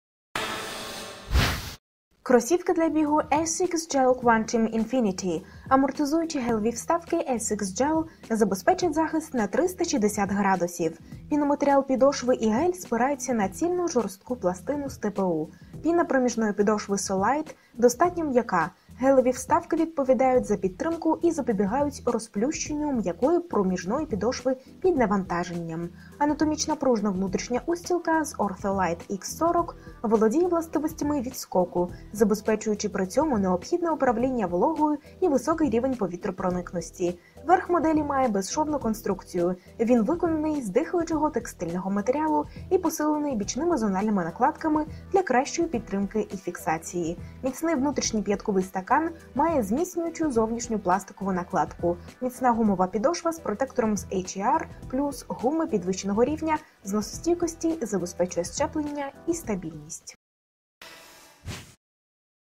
Кроссовки для бега Asics GEL-QUANTUM INFINITY BLK/ORNG M SS19 1021A056-002
Кросівки для бігу Asics GEL-QUANTUM INFINITY BLK / ORNG M SS19

Амортизуючі гелеві вставки ASICS GEL забезпечать захист на 360 градусів. Піноматеріал підошви і гель спираються на цільну жорстку пластину з TPU (термополіуретан). TRUSSTIC SYSTEM® TECHNOLOGY знижує вагу при збереженні структурної цілісності взуття. Пластикова пластина простягається від носка до п'яти. Решітчаста перемичка з'єднує обидві сторони пластини під серединою стопи. Під п'ятою пластикова пластина звужується до центру і спрямовується вгору. Це здійснює підтримку п'яткової зони, а під навантаженням, трохи згинаючись, працює за принципом автомобільної ресори. Піна проміжної підошви SOLYTE® достатньо м'яка, гелеві вставки і пластина з TPU надають пружність. Більш тверді, ніж піна, гелеві вставки відповідають за підтримку і запобігають "розплющенню" м'якої проміжної підошви під навантаженням. Амортизація відчувається по центру підошви, за рахунок того, що піноматеріал підошви м'якше зовнішнього кільця з гелю. Анатомічна пружна внутрішня устілка з ORTHOLITE® Х-40 володіє властивостями відскоку, забезпечуючи при цьому необхідне управління вологою і високий рівень повітропроникності. Верх моделі має безшовну конструкцію, він виконаний з дихаючого текстильного матеріалу і посилений бічними зональними накладками для кращої підтримки і фіксації. Міцний внутрішній п'ятковий стакан має зміцнюючу зовнішню пластикову накладку. Міцна гумова підошва з протектором з AHAR +, гуми підвищеного рівня зносостійкості, забезпечує зчеплення і стабільність. Кольорові рівні підошви покликані підкреслити фірмовий стиль ASICS. Asics GEL-QUANTUM INFINITY важать 380 гр. Перепад від п'яти до носка становить 10 мм. Модель підійде спортсменам з нейтральною або гіпопронацією стопи.

Технології: 360 GEL Cushioning; TPU Trusstic System; ORTHOLITE® Х-40; SOLYTE®; AHAR +
Матеріал: текстиль / поліуретан / піноматеріал
Колір: чорний / жовтий / помаранчевий